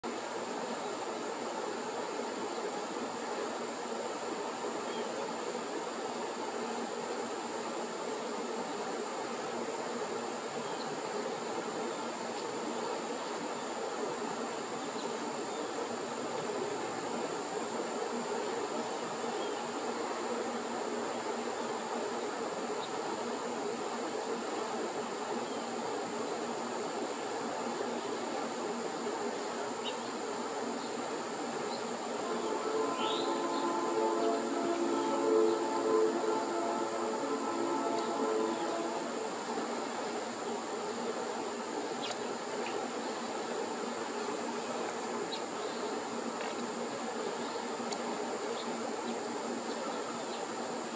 Hummingbird slurping nectar, in Forest Park, Birmingham, Alabama,
Outside the home of Tom Gordon,in  Birmingham, Alabama, hummingbirds are stopping frequently to drink nectar from a feeder hanging from a hardwood tree. Hummers appear more frequently in Gordon's Forest Park neighborhood in late July and August.